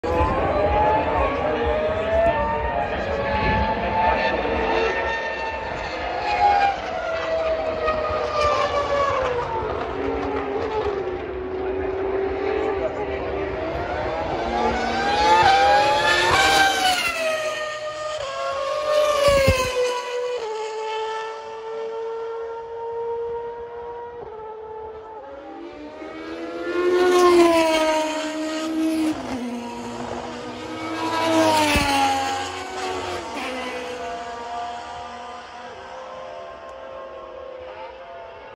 Legends Parade Red Bull Ring Spielberg 10.7.2022 FW25 RB8
Mathias Lauda, Ferrari 312B3 (1974 / Niki Lauda)
Zak Brown, Lotus 79 (1978 / Mario Andretti)
Riccardo Patrese, Brabham BMW BT52 (1983)
Martin Brundle, Benetton B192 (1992)
Ralf Schumacher, BMW Williams FW25 (2003)
David Coulthard, Red Bull RB8 (2012 / Sebastian Vettel)